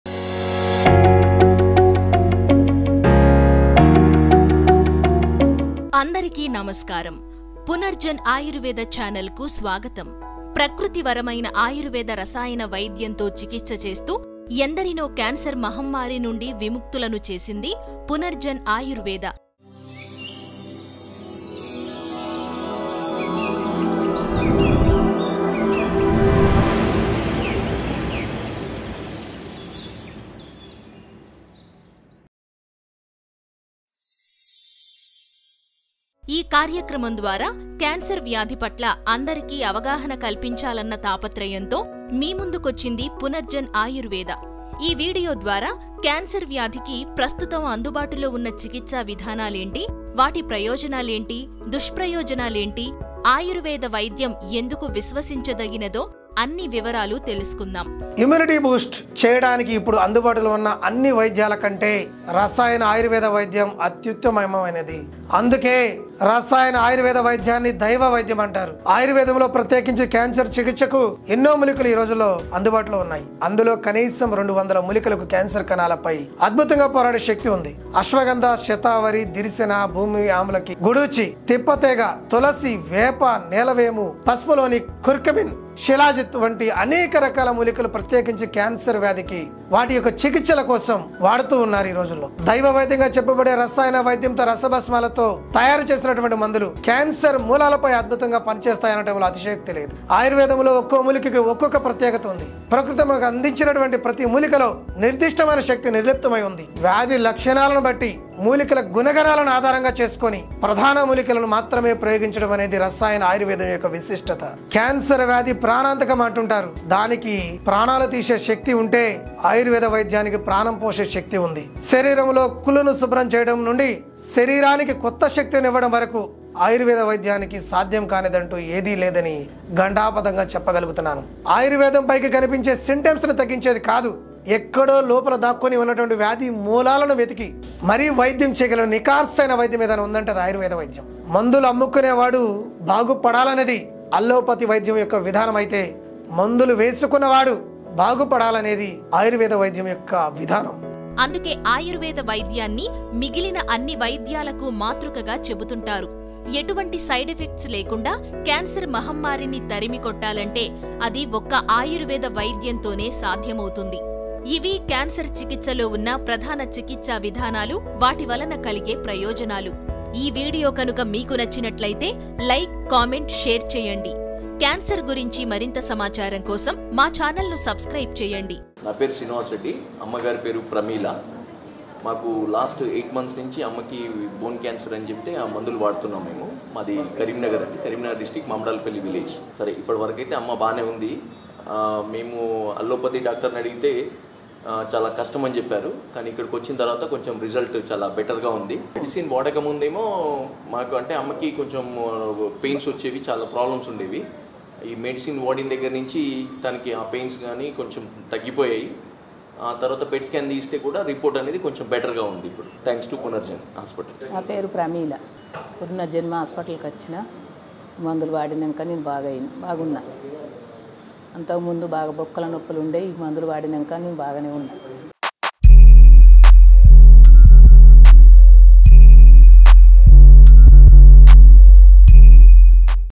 Importance of Rasayana Ayurveda in Telugu | Bone Cancer Survivor Testimonial | Punarjan Ayurveda
Importance of Ayurveda in Telugu | Benefits of Ayurvedic Treatment in Telugu | Punarjan Ayurveda